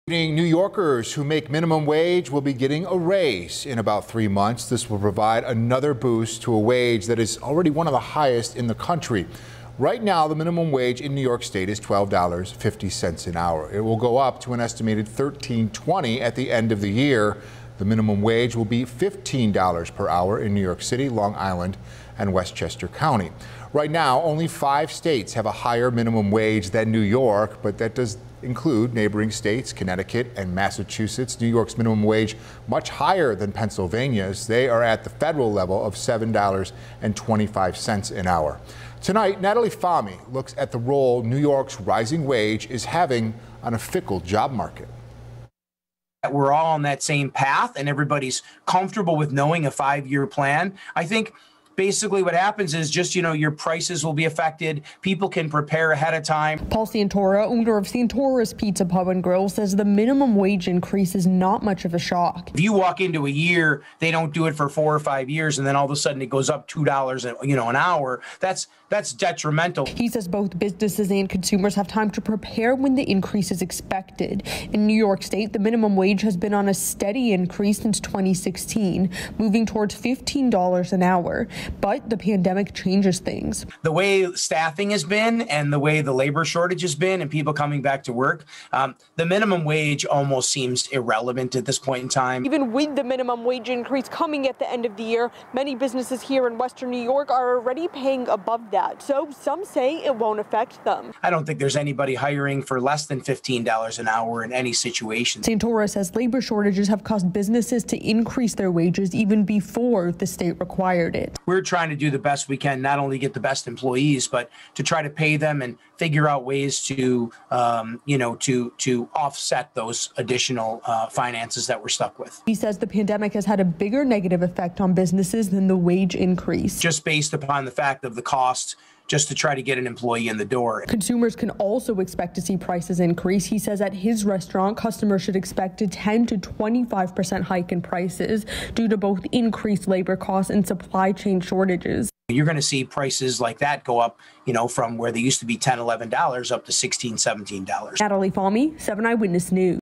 Minimum wage in WNY will increase by $.70 at the end of the year
Both businesses and consumers have time to prepare when the increase is expected. In New York State, the minimum wage has been on a steady increase since 2016, moving towards $15 an hour. But the pandemic changes things.